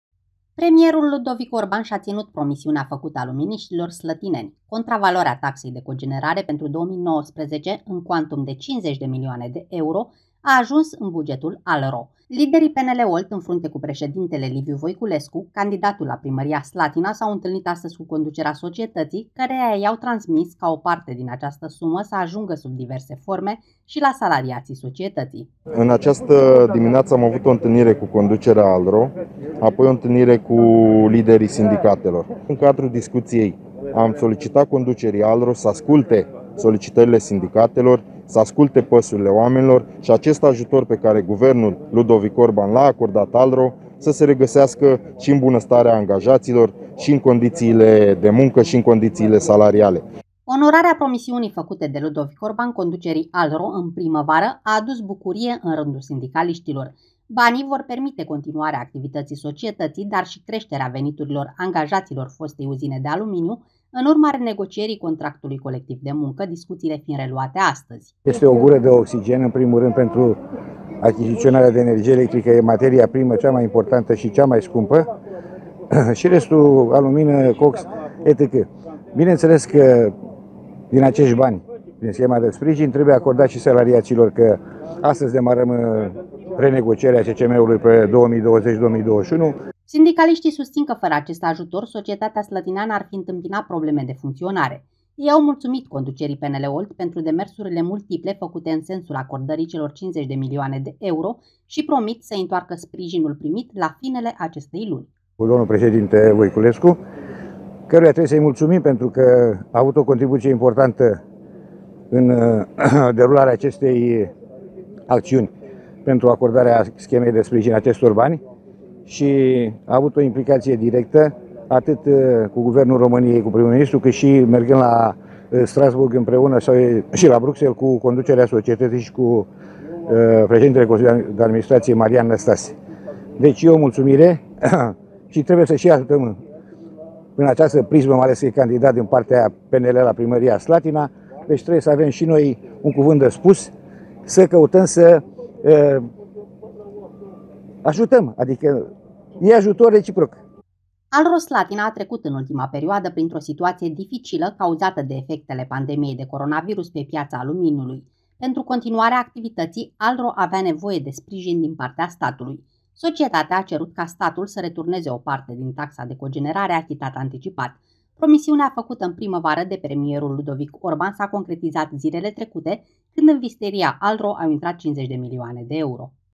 Promisiunea onorată de guvernul Orban crește salariile angajaților ALRO
Banii din taxa de cogenerare returnați Alro de guvern, conform promisiunii făcute în primăvară de premierul Ludovic Orban, deblochează discuțiile dintre sindicaliști și conducerea societății pentru renegocierea contractului de muncă. Conducerea PNL Olt, în frunte cu liderul Liviu Voiculescu, candidatul la Primăria Slatina, s-a întâlnit astăzi, atât cu staff-ul Alro, cât și cu liderii sindicatelor din cadrul societății. Liderul liberalilor olteni a solicitat conducerii Alro ca o parte din cele 50 milioane de euro primiți să se regăsească și în creșterile salariale ale angajaților, dar și în creșterea gradului de confort la locul de muncă.